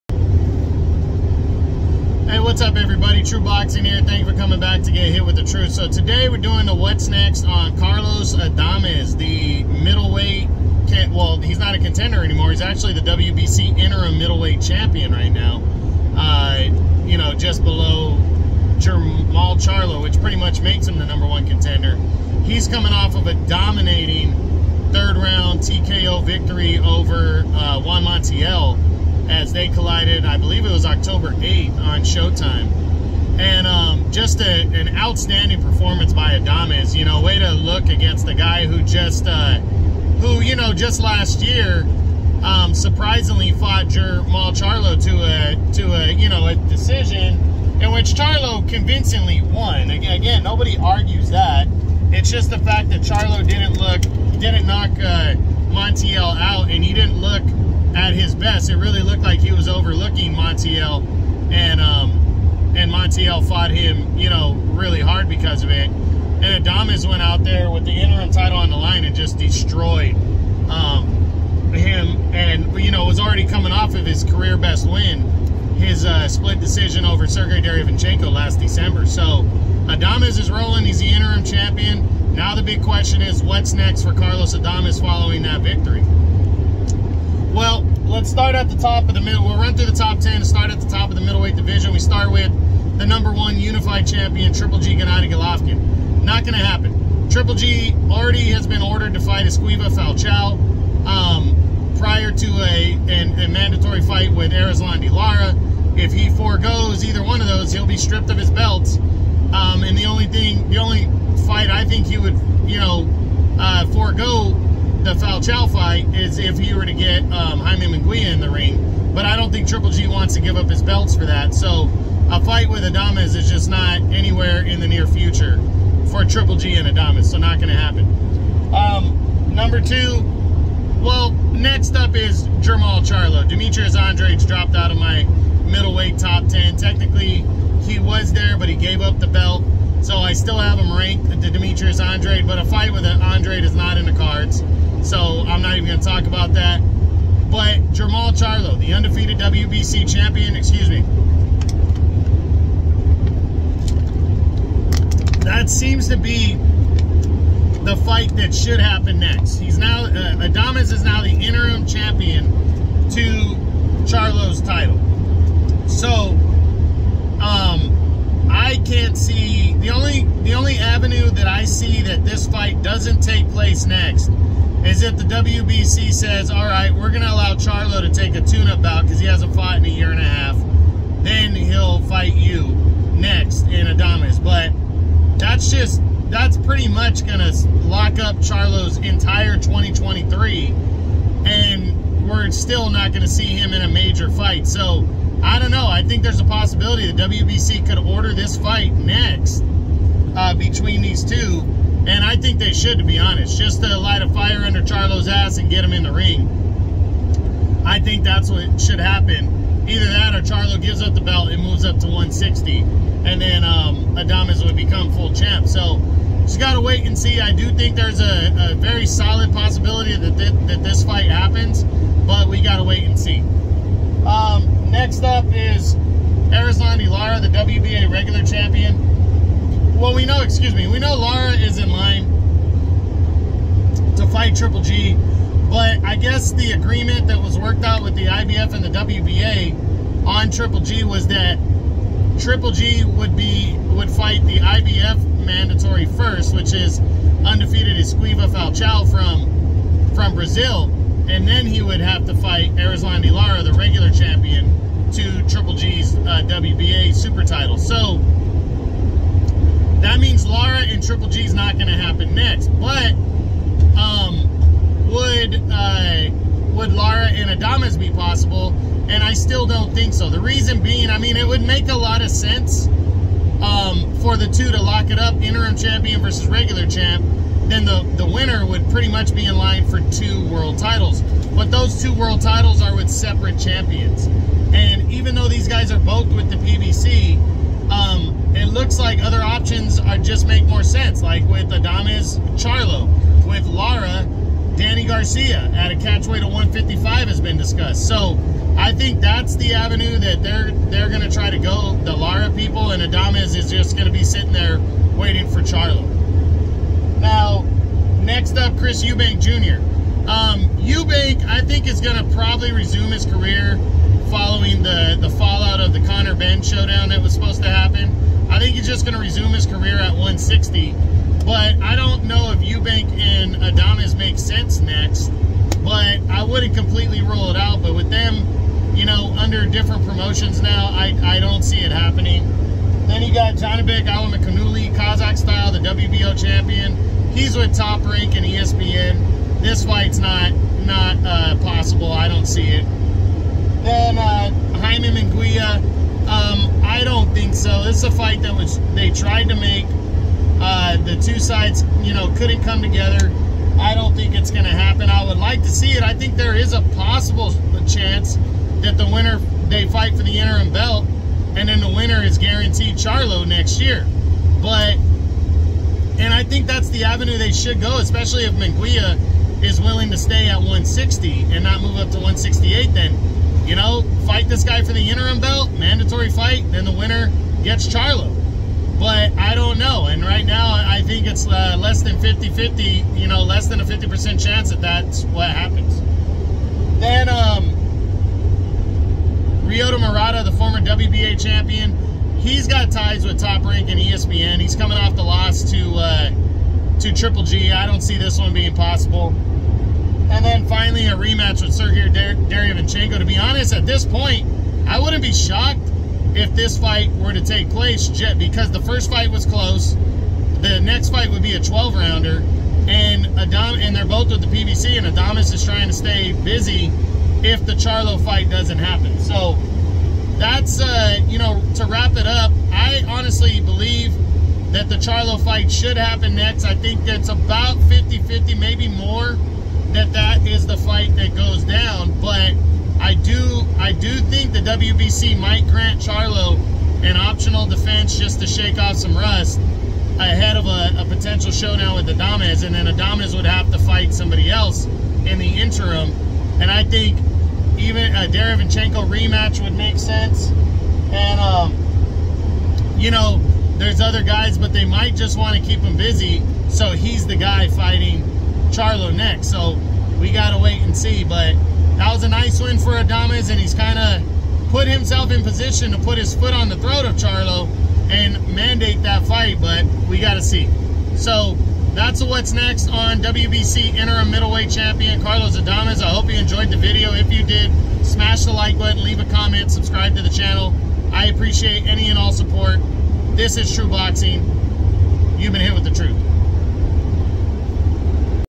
What's Next? Carlos Adames
What's possibly coming up for the interim Middleweight Champ following a dominant TKO win over Juan Montiel